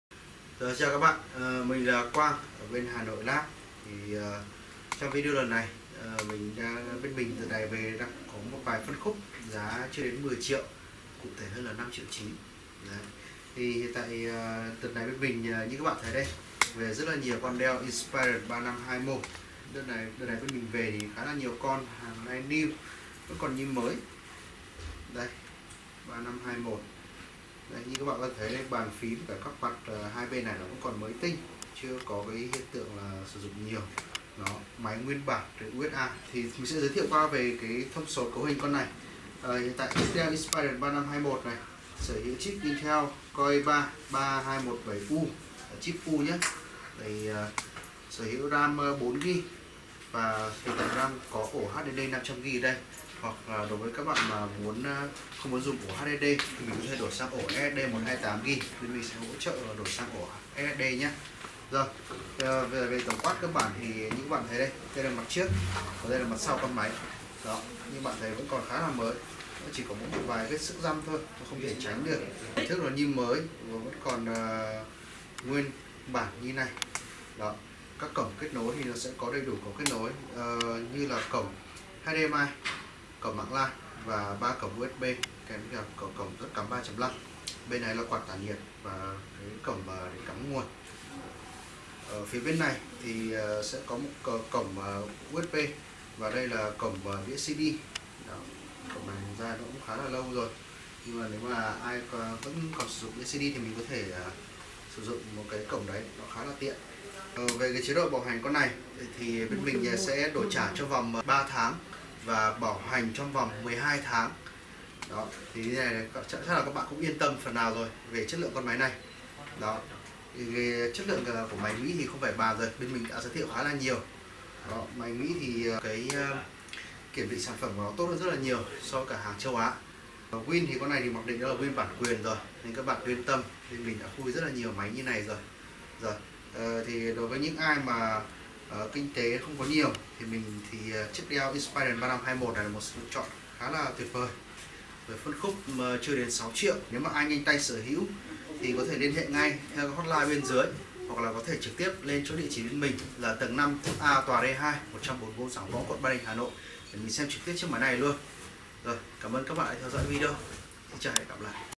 HANOILAB: Giới thiệu Dell Inspiron 15R 3521 USA nguyên bản
HANOILAB: Giới thiệu Dell Inspiron 15R 3521 USA nguyên bản giá cực rẻ chỉ 5tr9 !!!

CẤU HÌNH : Intel i3-3217U / Ram 4G/ 500GB HDD hoặc 128GB SSD / Win10 Pro / 15 INCH
Đổi trả trong 3 tháng đầu nếu máy có lỗi từ NSX